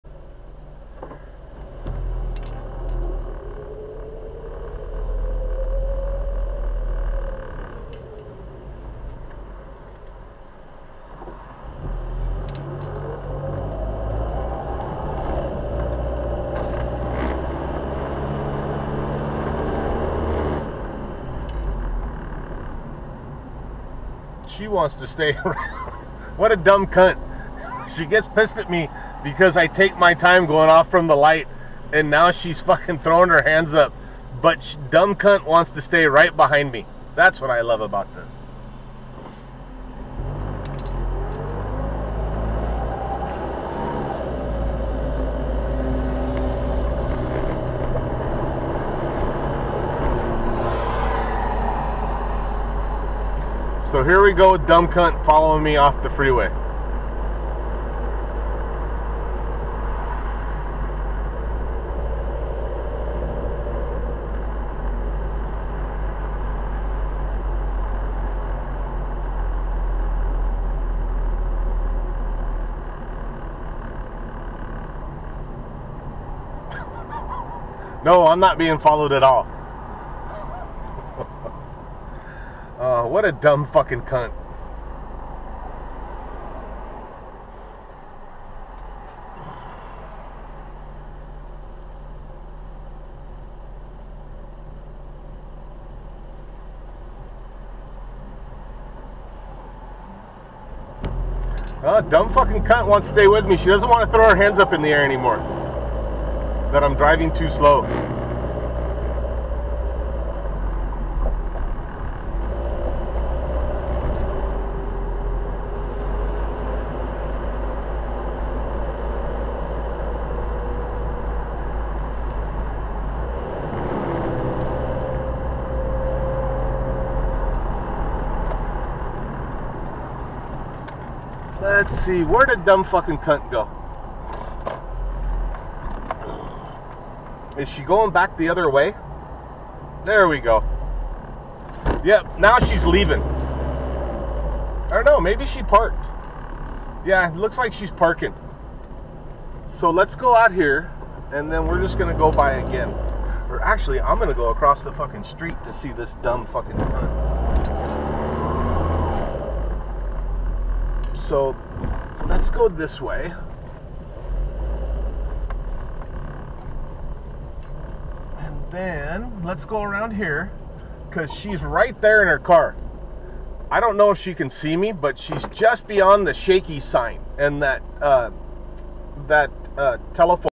Valley View exit from 22 west FW white Toyota Part 3
North on Valley view and she still wants to stay behind me, throwing her hands up in the air for my rear camera.